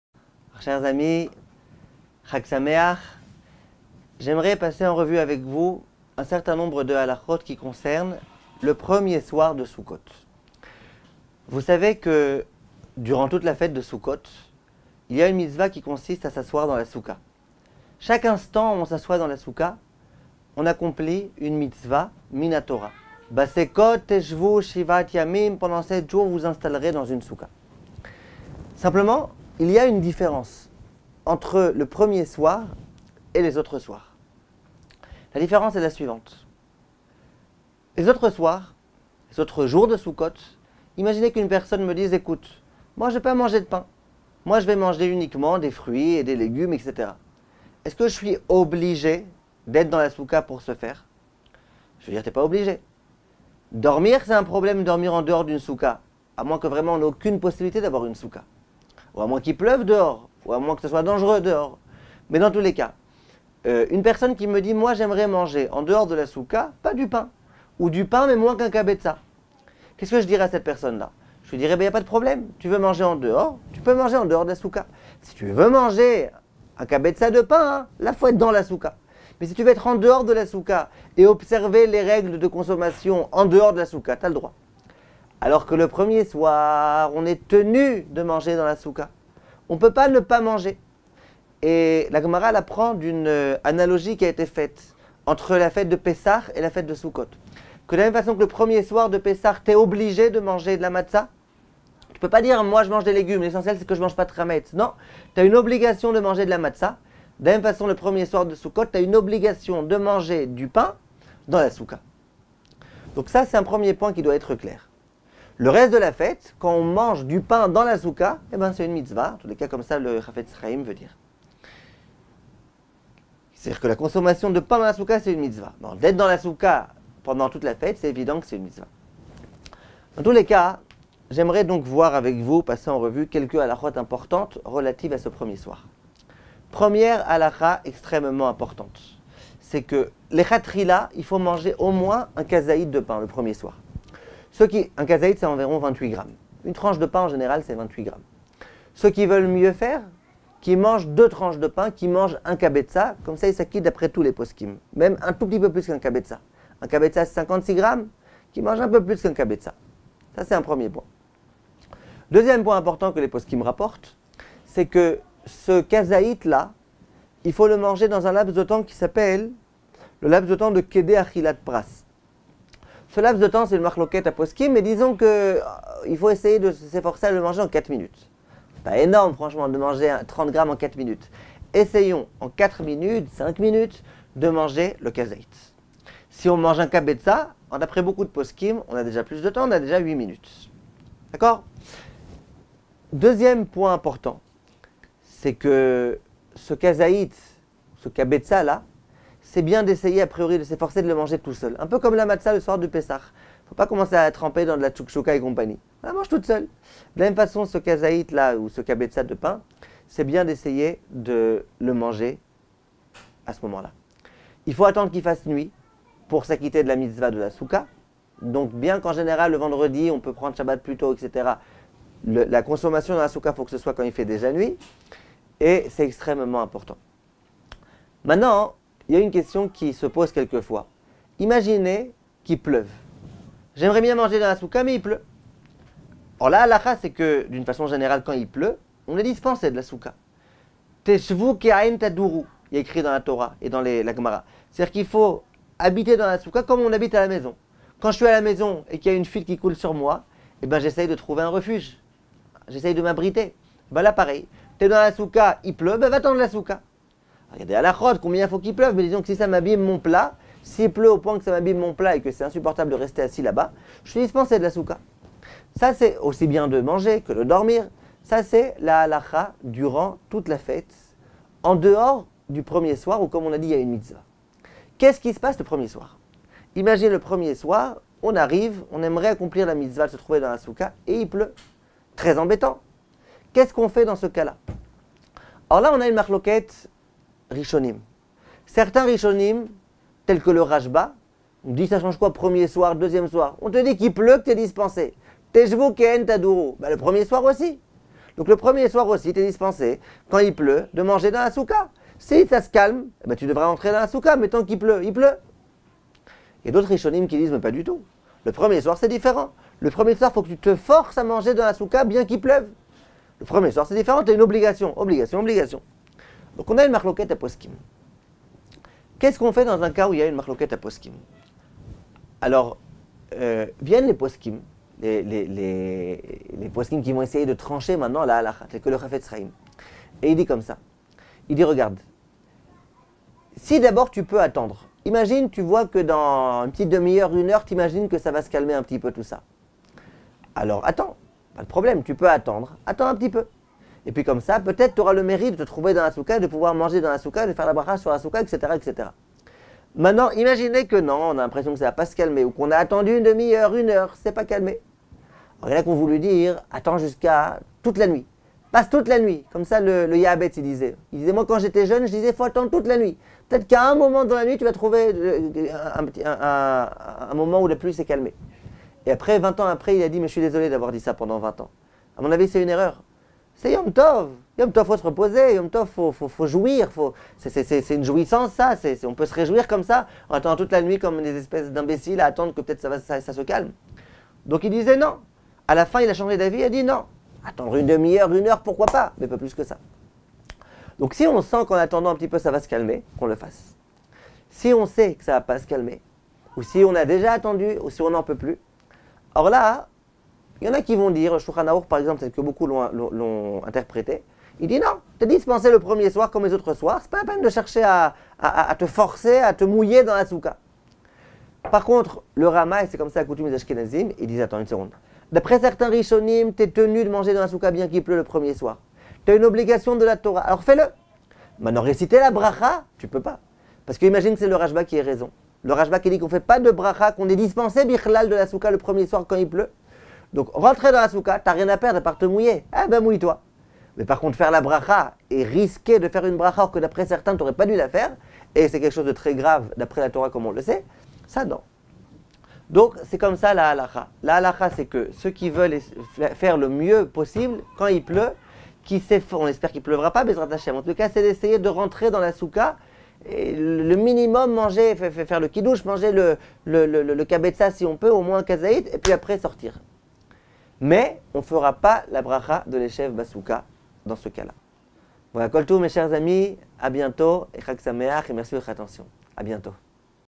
Rav Azriel Cohen-Arazi - Les lois essentielles du premier jour de Souccot
Cours enregistré le 28 septembre 2023